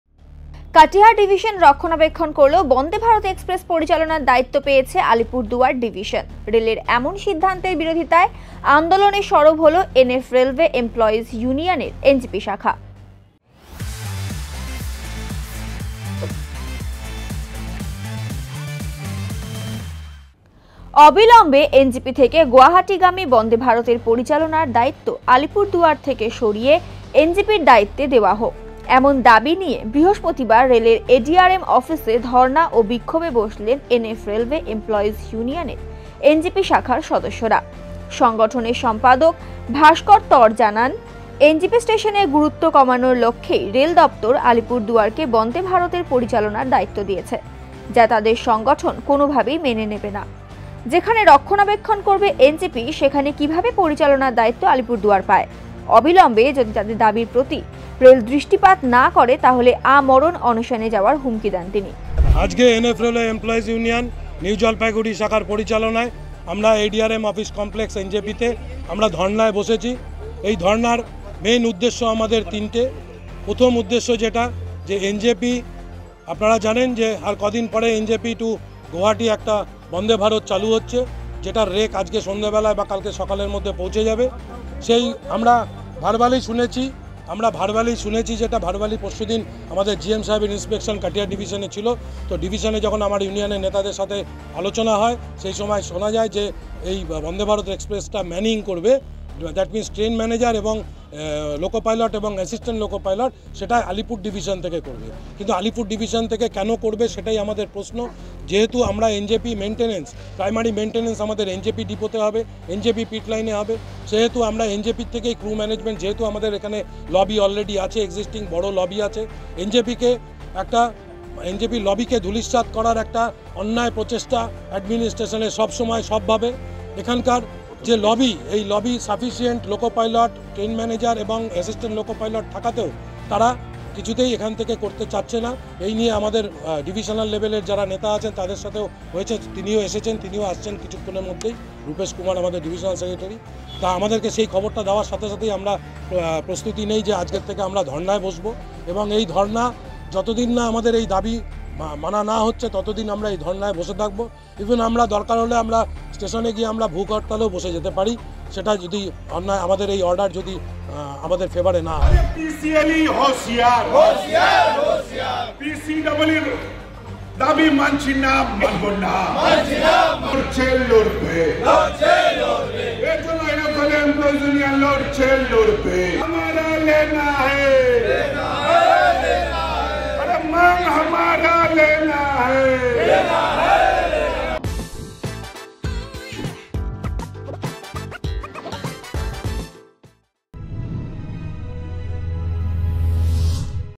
Railway Employees Union protest against Alipurduar Division operating Vande Bharat Express (Bangla)
NF Railway Employees Union raises voices in protest against Alipurduar Division's responsibility for operating Vande Bharat Express (Bangla)